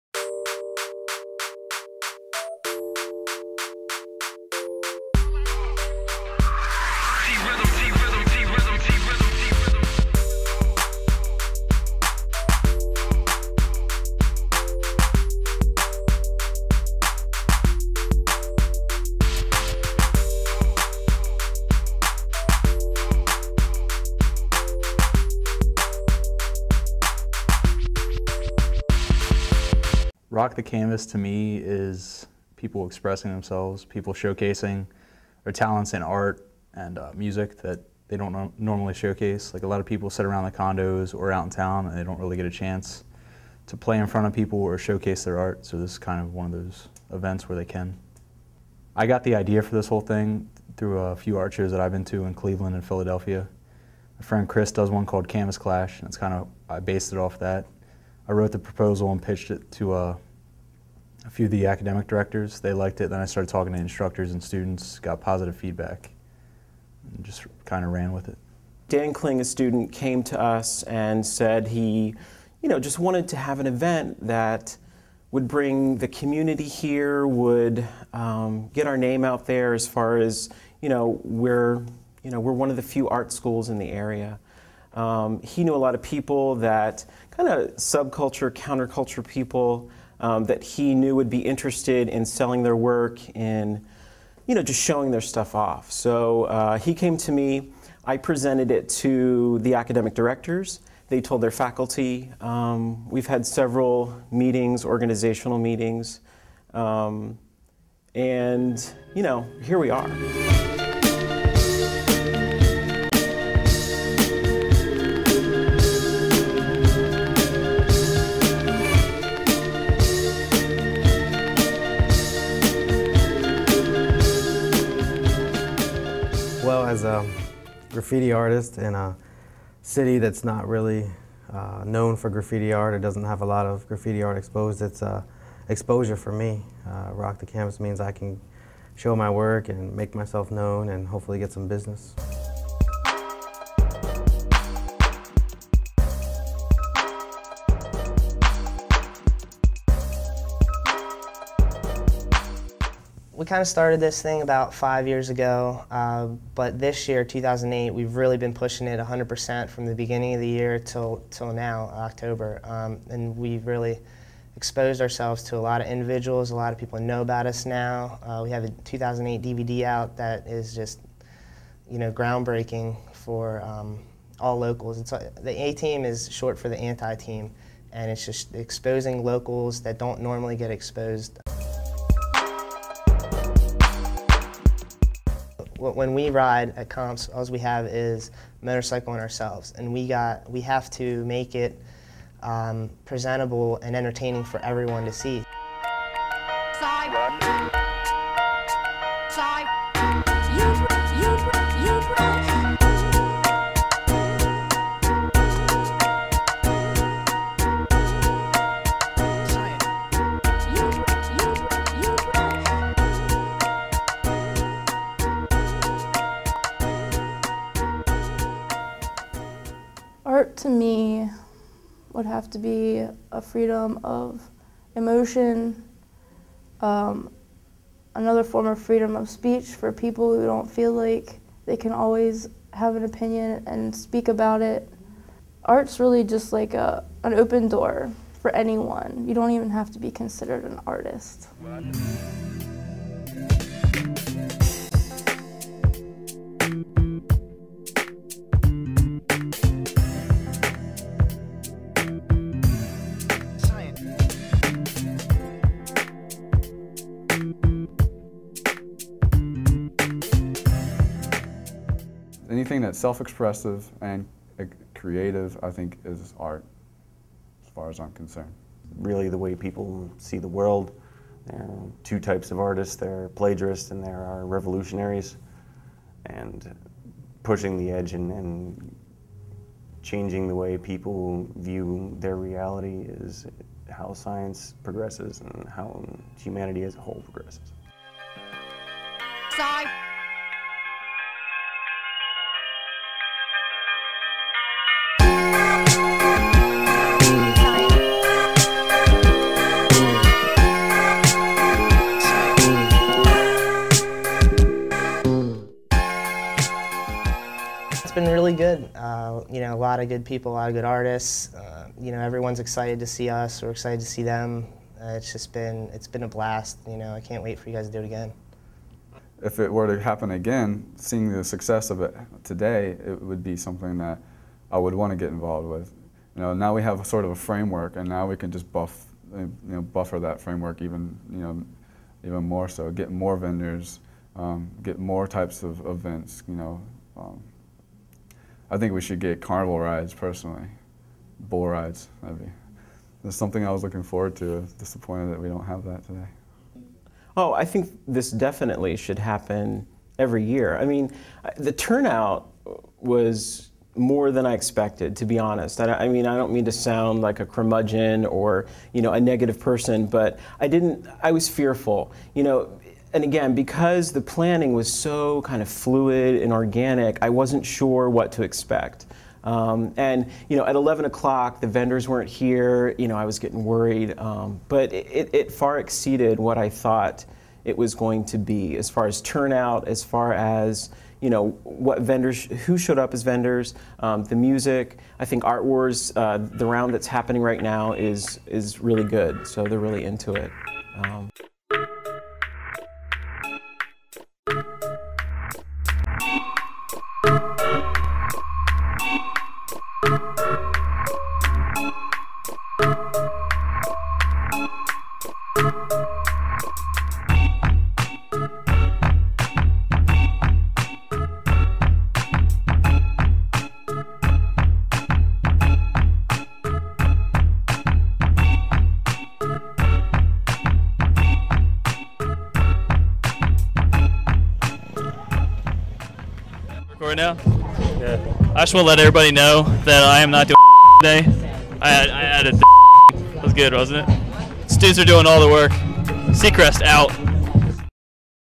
Rock The Canvas - Documentary
Short documentary on the first time event entitled Rock the Canvas held at The Art Institute of York - PA. Rock the Canvas was basically a showcase of the Hip-Hop culture: BBoying, DJing, Graffiti, and Rapping. Along with a showcase of art and street bikes.